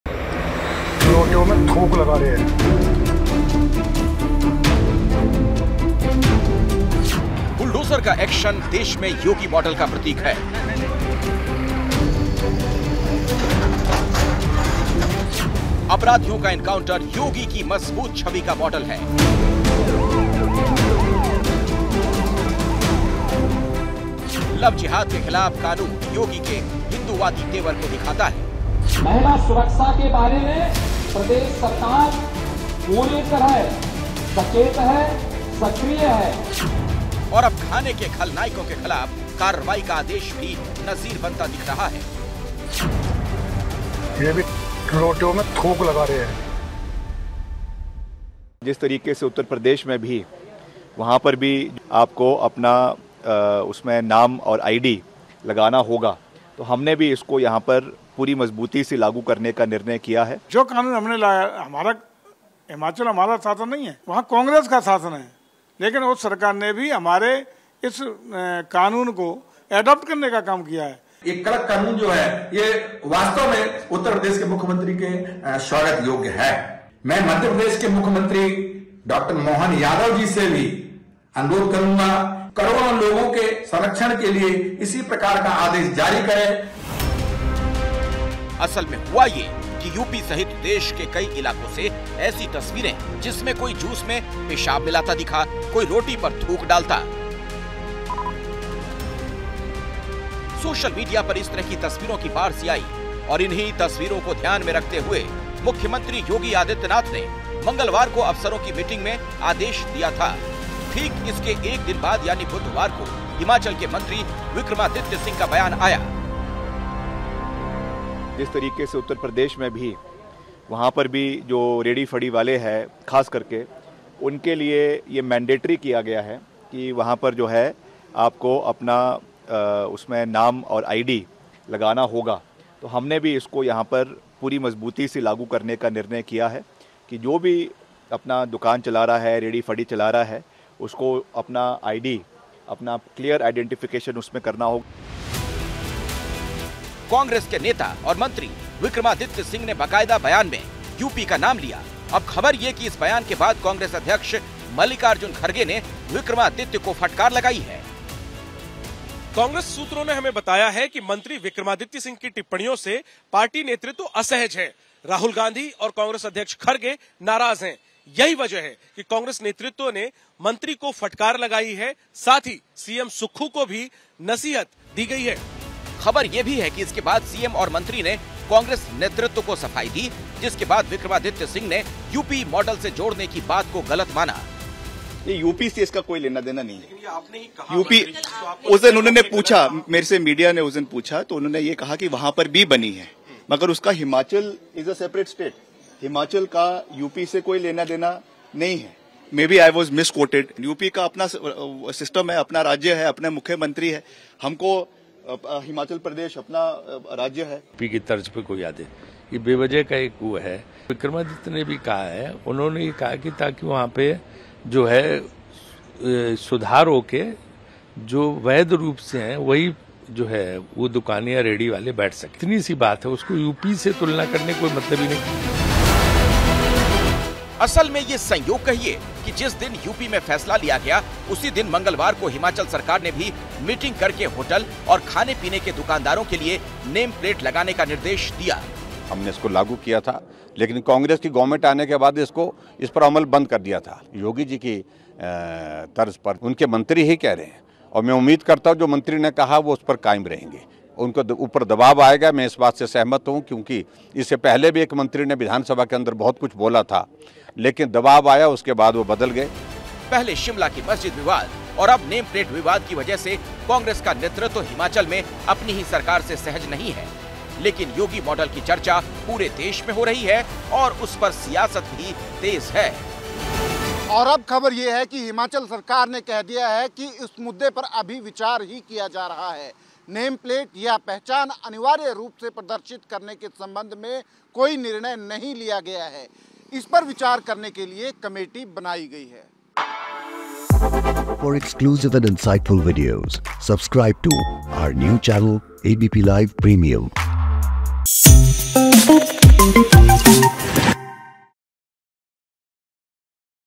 Himachal Pradesh में योगी मॉडल को लेकर कांग्रेस में हड़कंप..मंत्री विक्रमादित्य को आलाकमान की फटकार?
बात हिमाचल प्रदेश की... जहां मंत्री विक्रमादित्य को कांग्रेस नेतृत्व की फटकार लगने की खबर है..बताया जा रहा है नेम प्लेट विवाद से आलाकमान नाराज है... और अब राज्य सरकार इस पर सफाई देती नजर आ रही है... पूरी खबर देखिए